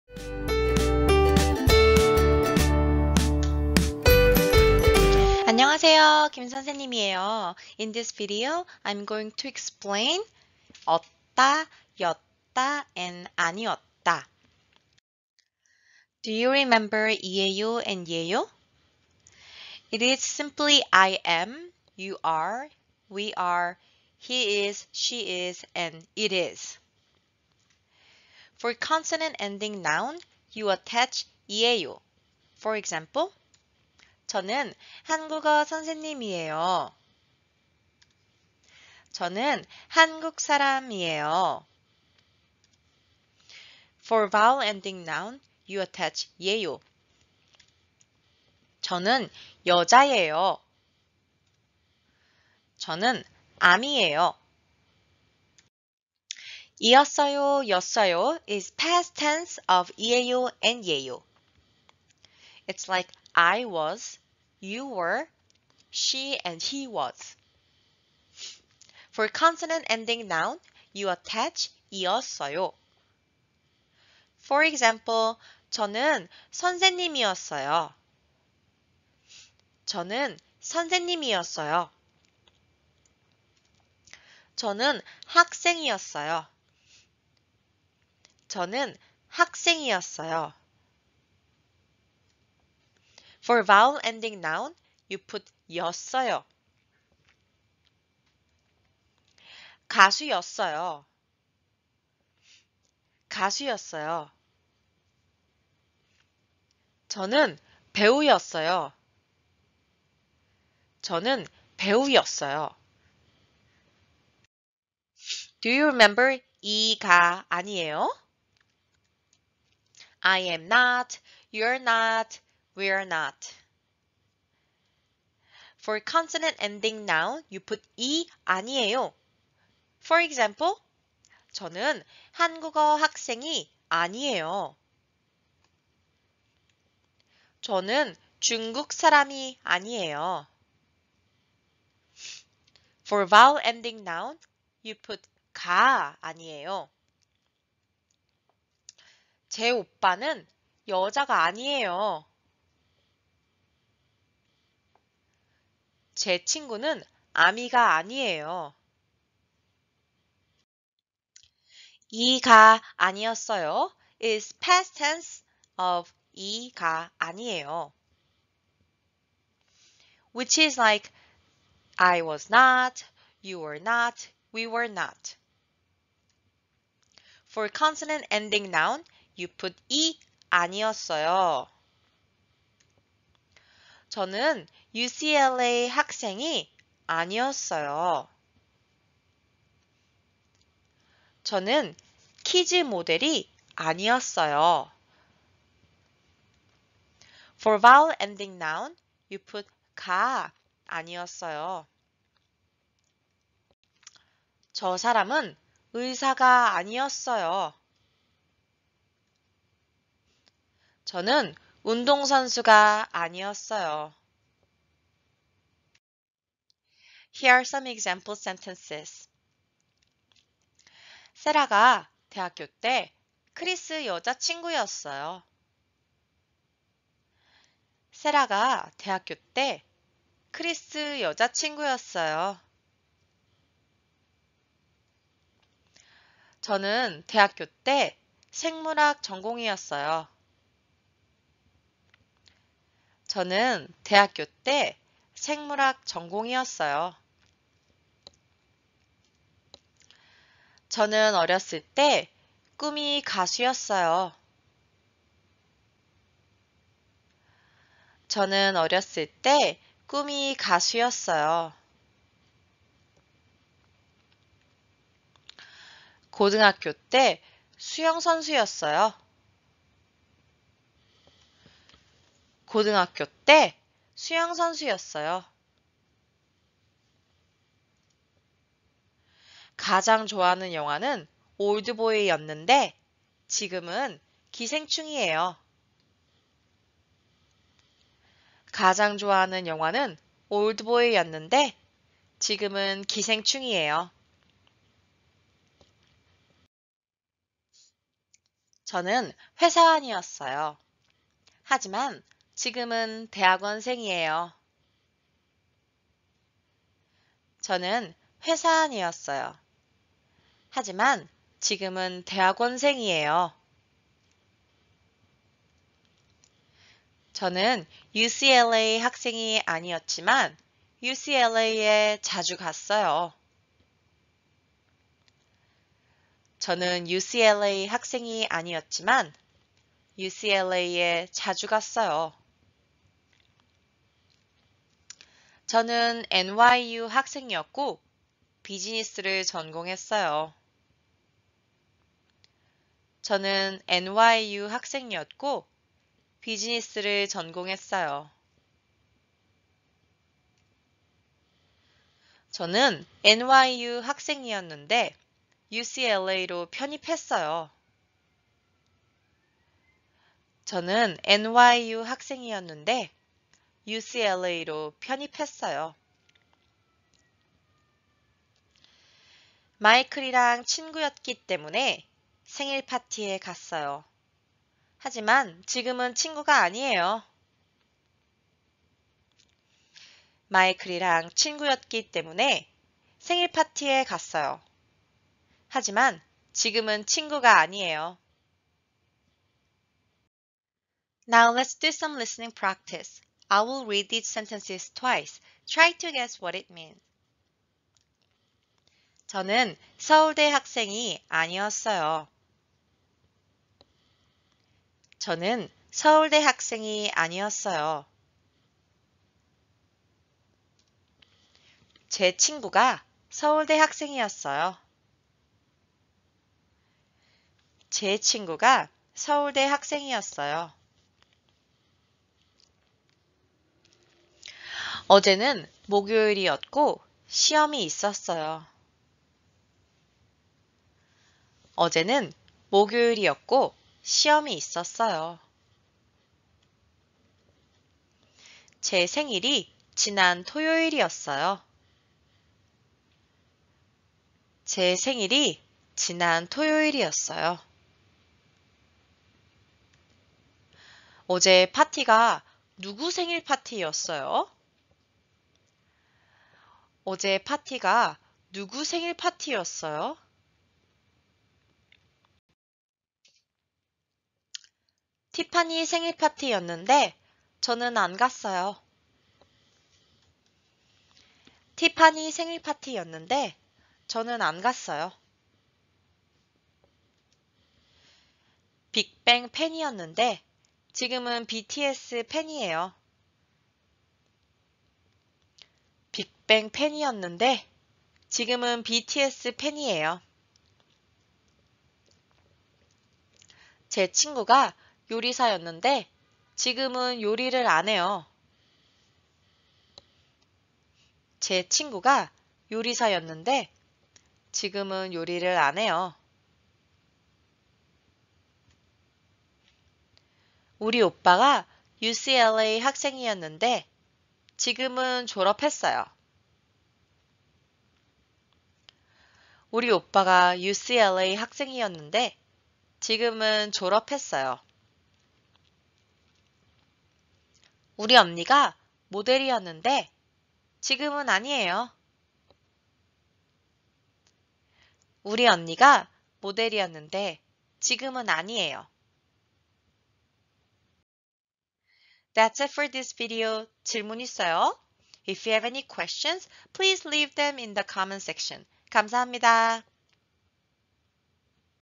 n었다, 였다, 아니었다 grammar / Beginning Level / 한국어 연습해요 / 듣기 연습 / For Korean language learners
Practice Korean with me! This video explains n었다, 였다, 아니었다 grammar (past tense of 이에요/예요/아니에요) with examples.
Repeat after me! Pause, slow, or re-watch the video if needed. 😊

-김 선생님